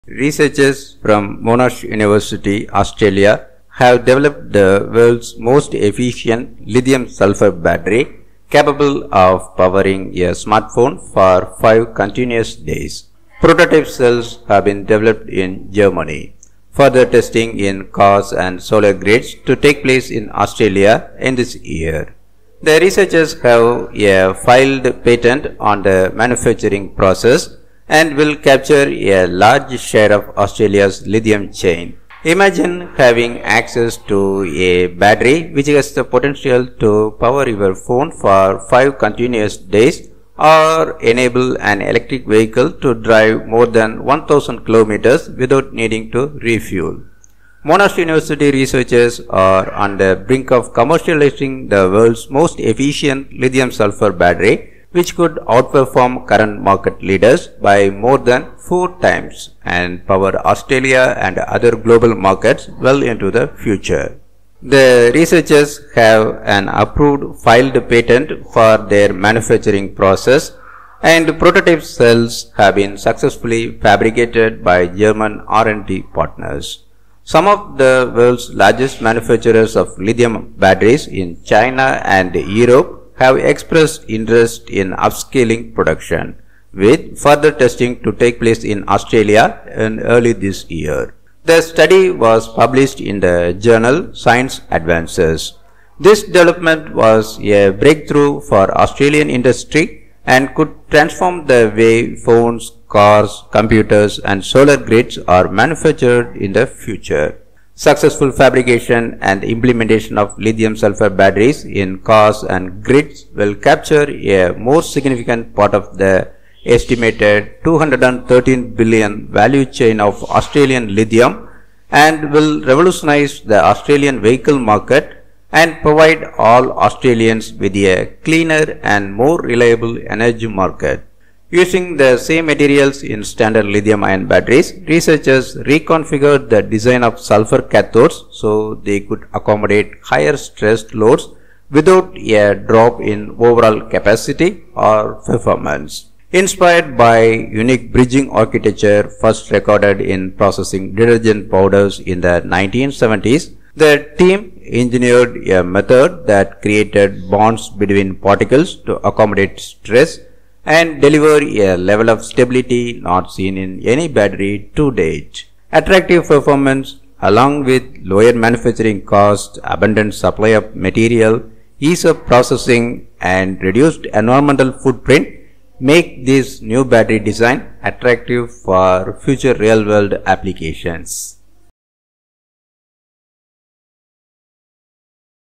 New Lithium-sulphur battery is capable of powering a smartphone for 5 days
Researchers from   Monash University, Australia have developed the world's most efficient lithium-sulphur battery, capable of powering a smartphone for five continuous days.
Prototype cells have been developed in Germany. Further testing in cars and solar grids to take place in Australia in  this year.
The researchers have a filed patent on the manufacturing process, and will capture a large share of Australia's lithium chain.

Credit: Monash University